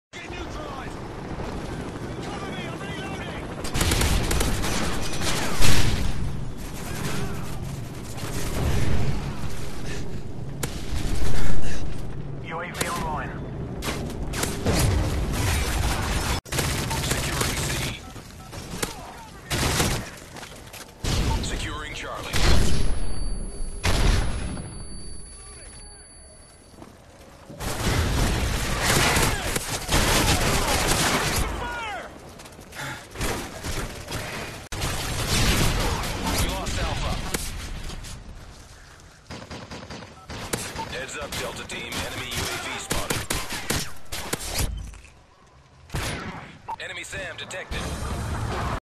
Knifing Juggs
Little collection of 3 Juggs I've ended with a knife, hopefully more to come.